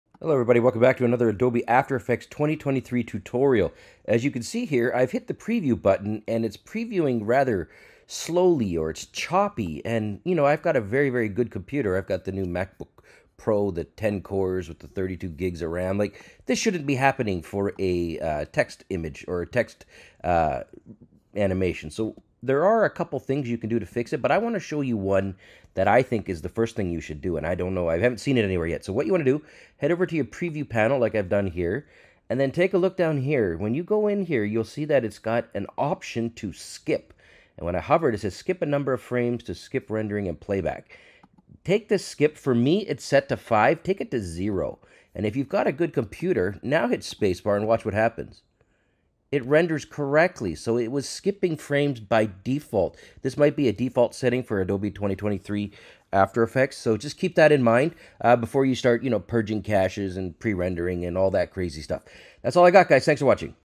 After Effects 2023 - How To Fix Slow Preview and Lag - Potential Fix
In this quick tutorial I show you how to potentially fix slow preview or laggy playback in Adobe After Effects 2023.  The first step is to go to the preview panel and change the skip from 5 to 0.  This tells After Effects to stop skipping frames during playback, making the preview much faster and smoother.  This may not work as well on older computers.